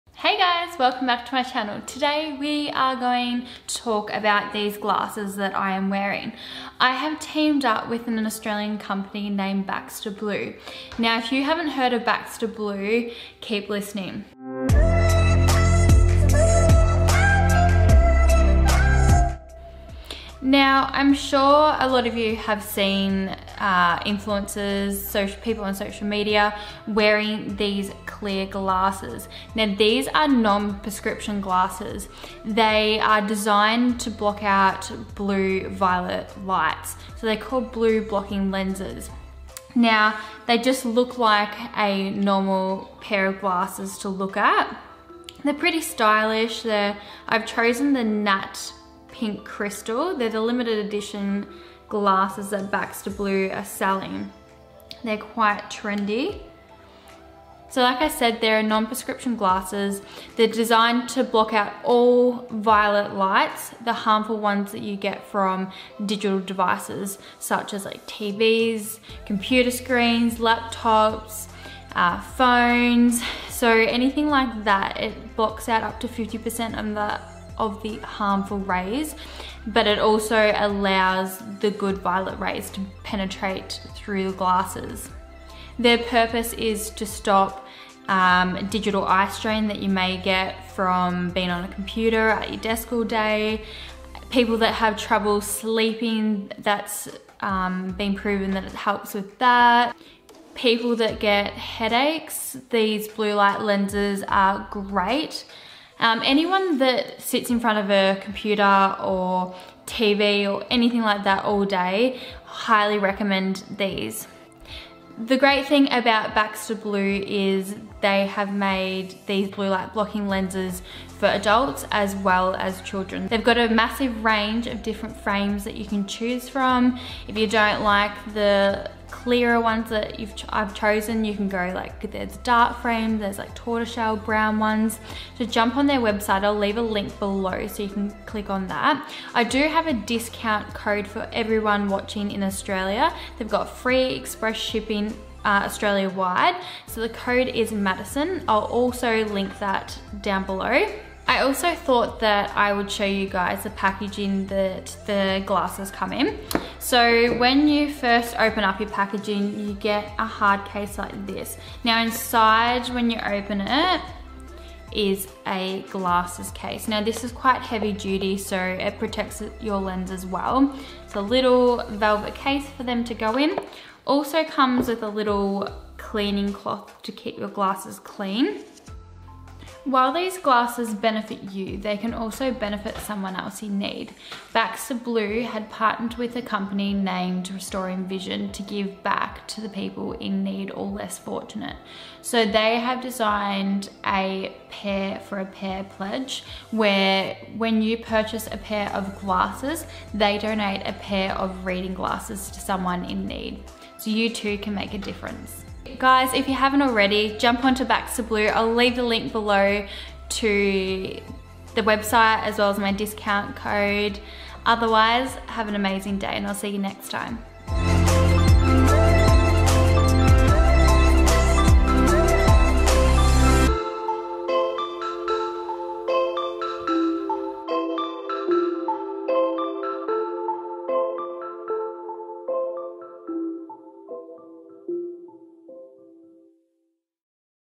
BAXTER BLUE - BLUE LIGHT BLOCKING LENSES (REVIEW)
Hi guys, today I'm reviewing the Baxter Blue glasses. They are an Australian company doing amazing things.

‣ USE CODE - MADDISON for free express shipping within Australia.